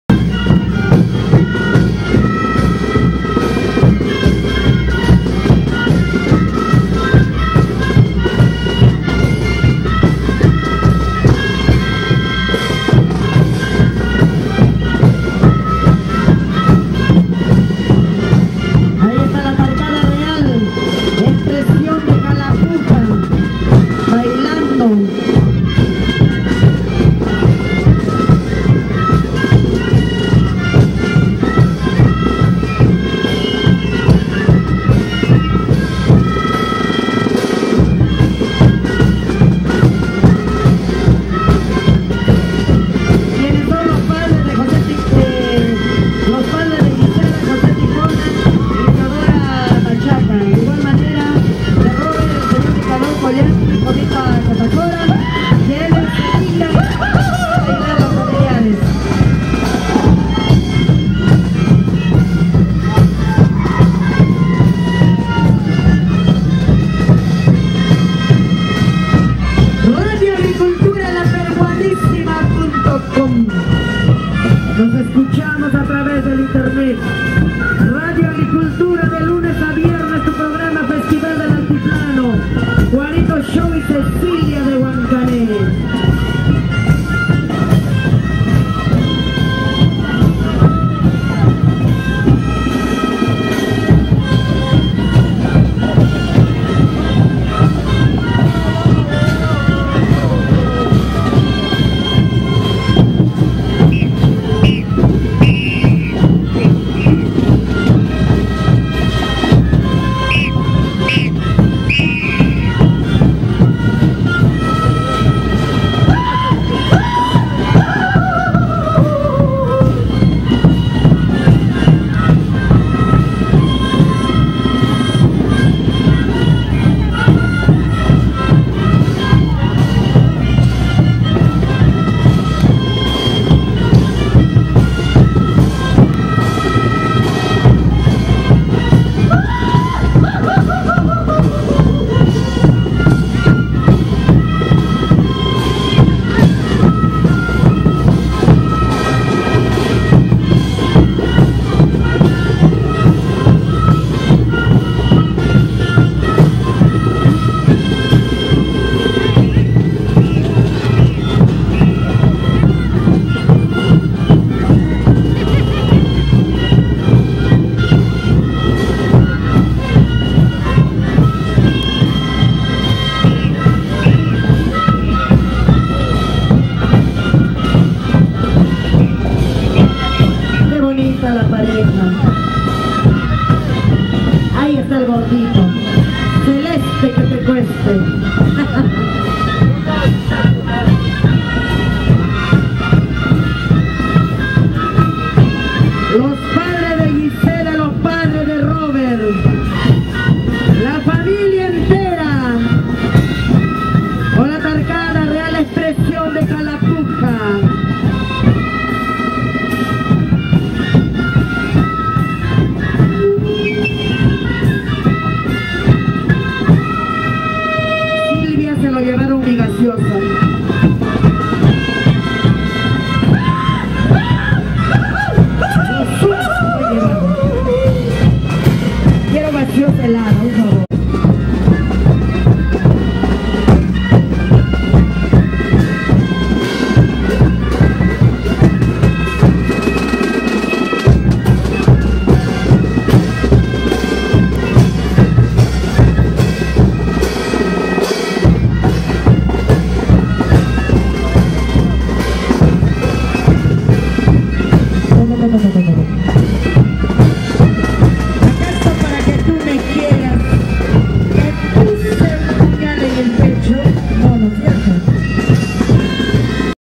REAL EXPRESION DE CALAPUJA
AHY LA FAMILIA  DE NICANOR CCOLLANQUI ENTRANDO CON SU PROPIA TARKEADA REAL EXPRESION DE CALAPUJA
REALIZADO EN EL STADIO MONUMENTAL DE COOP HUANCAYO--EL AGUSTINO 2011
ORGANIZA SUPER AMIGOS DE CALAPUJA